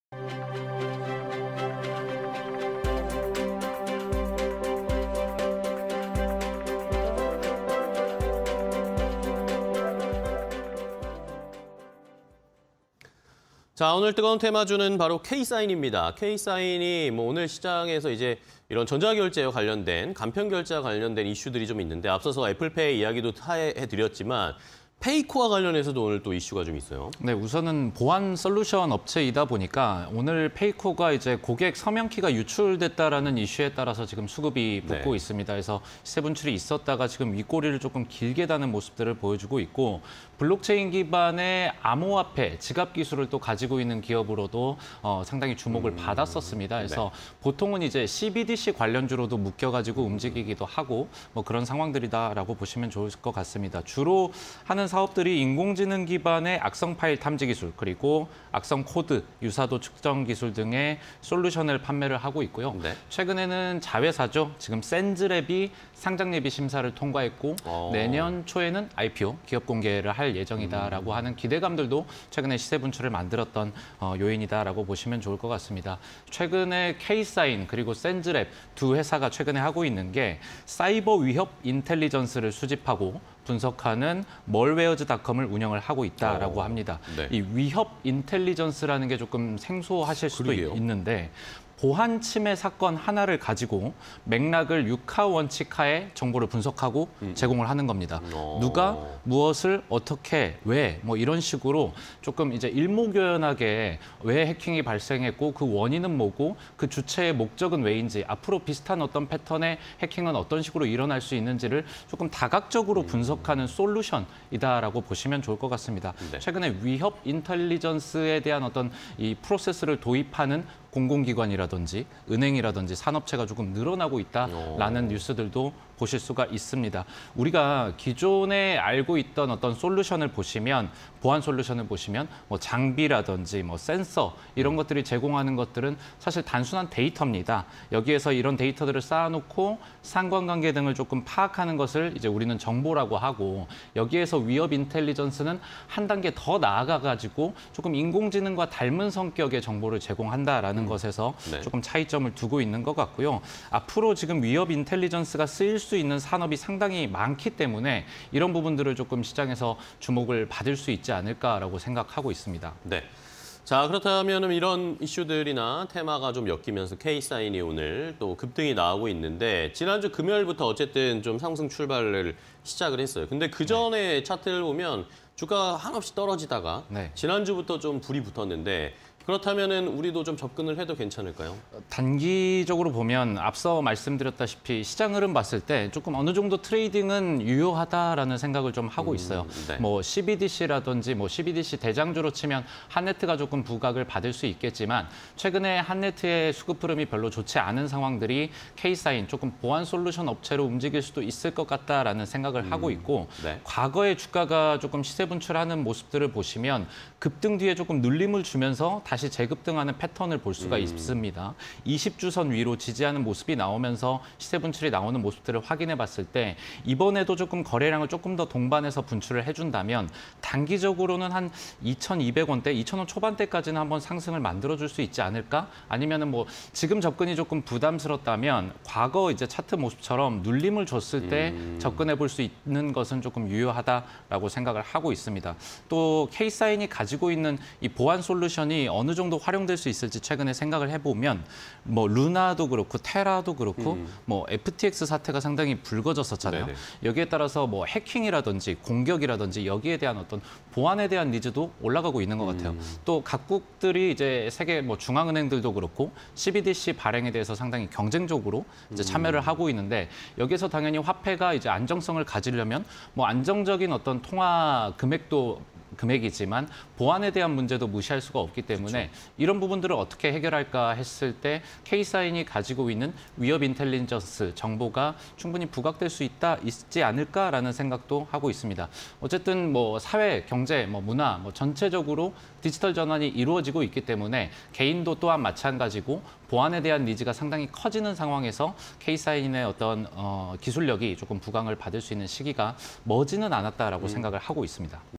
케이사인(192250)... '페이코' 고객 서명키 유출 소식에 강세_테마IN이슈 (20221206)
케이사인(192250)
- '페이코' 고객 서명키 유출 소식에 강세
- 블록체인 기반 암호화폐 전자지갑 기술 보유
- 자회사 '샌즈랩' 내년 초 IPO 예정